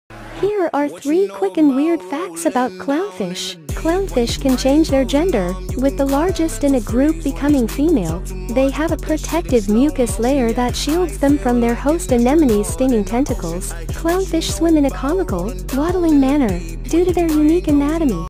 3 Weird Facts About Clownfish
Here are three quick and weird facts about clownfish:

1. Clownfish can change their gender, with the largest in a group becoming female.
2. They have a protective mucus layer that shields them from their host anemone's stinging tentacles.
3. Clownfish swim in a comical, waddling manner due to their unique anatomy.